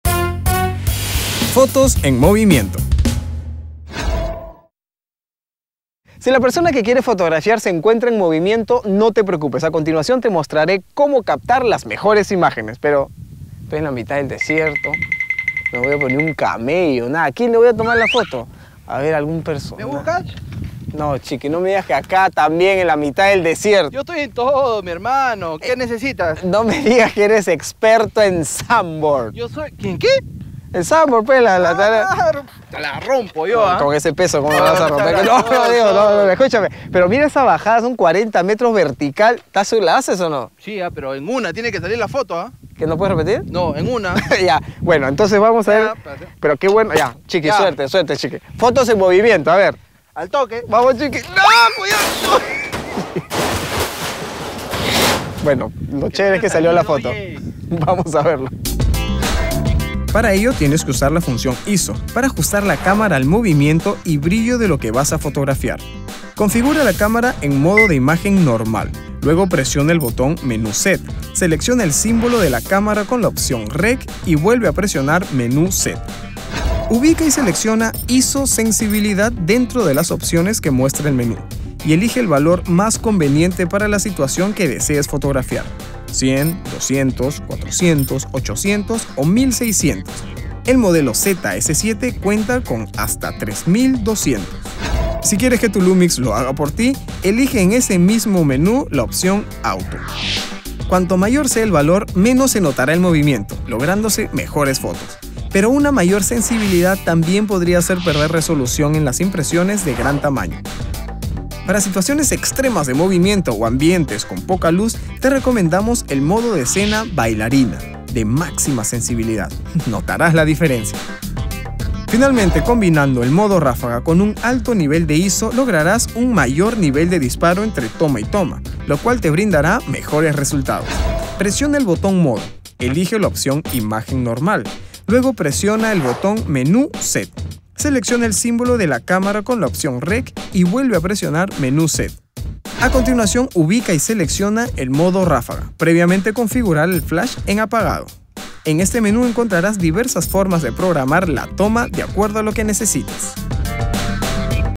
Cap. 4: Fotos en movimiento
Panasonic y Bruno te enseñan como hacer que tu foto salga perfecta con objetivos en movimiento.